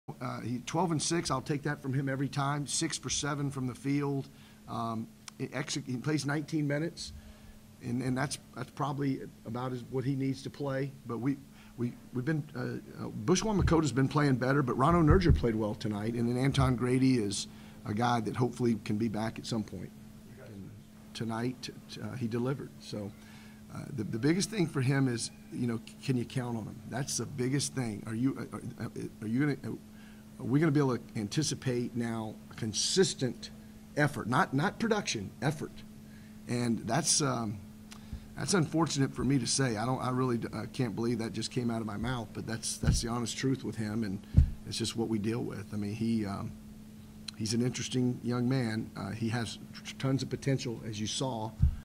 Shaq Morris' Role
This video is about Shaq Morris' role on this year's Wichita State team.